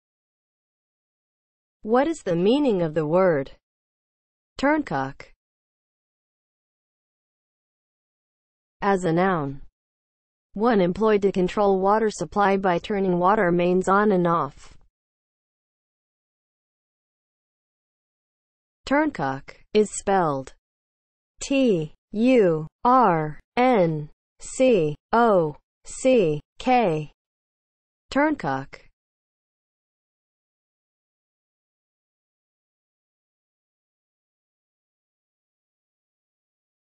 What is the meaning of the word TURNCOCK?
Definition and spelling of the word TURNCOCK.

(00:00) Intro
(00:07) As a noun
(00:17) Spelling

As a noun (00:07):
• one employed to control water supply by turning water mains on and off